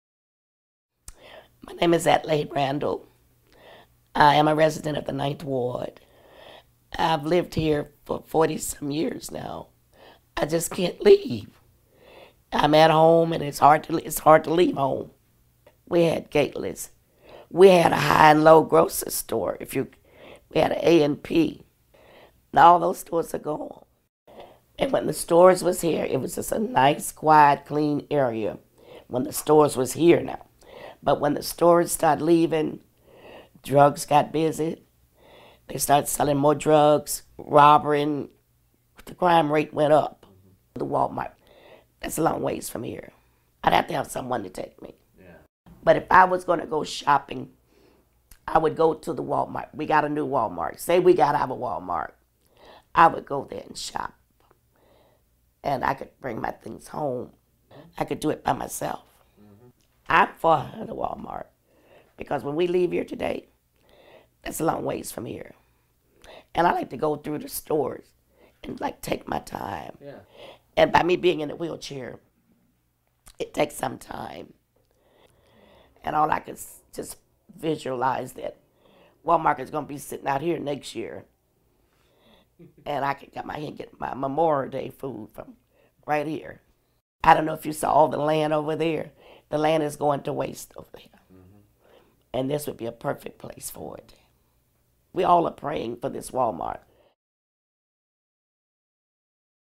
'We all are praying for this Walmart'
Adelaide Randall, resident of Chicago's 9th Ward, talks about how a Walmart in Pullman Park would help revitalize a neighborhood devastated by lack of jobs and lack of access to fresh groceries.